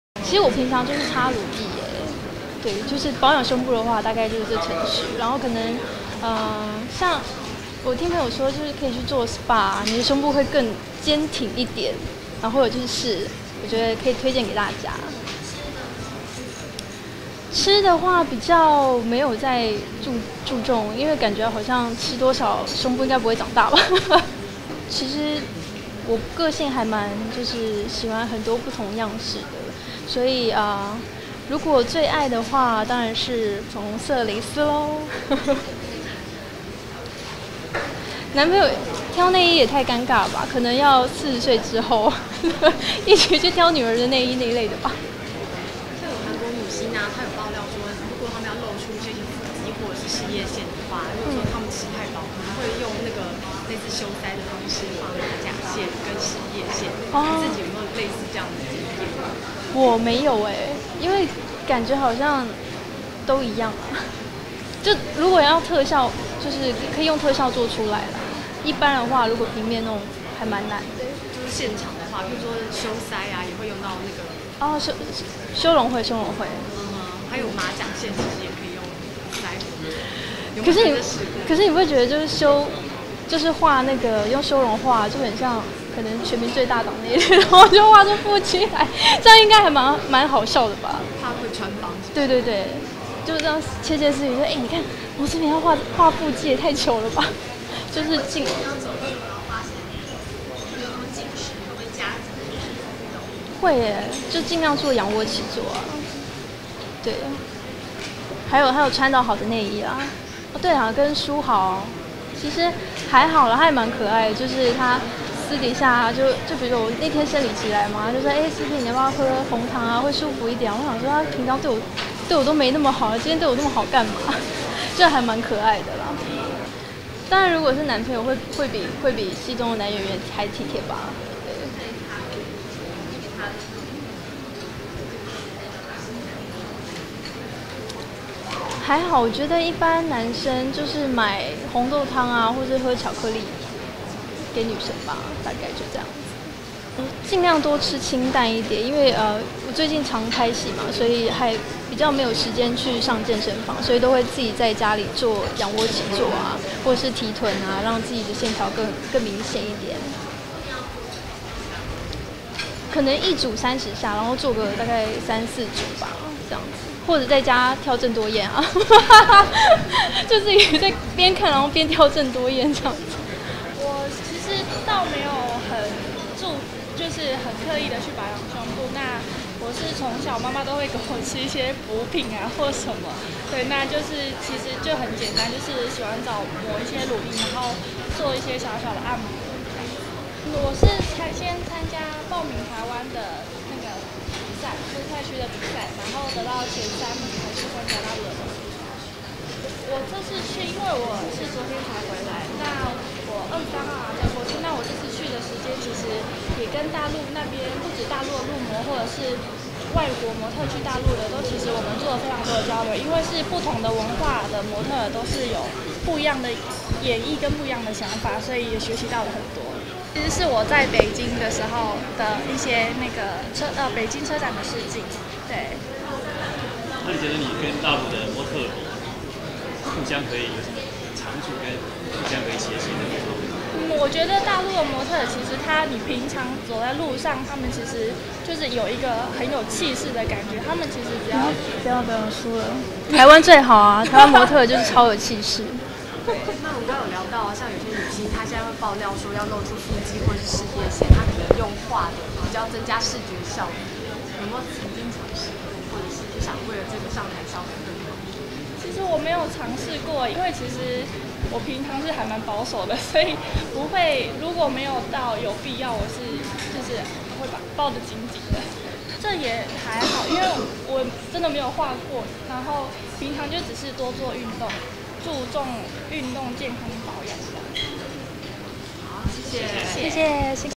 王思平 吳宜樺代言內衣 媒體聯訪
王思平 吳宜樺內衣代言，HOLD得住眾人目光、 帶動深V豐潮，媒體聯訪。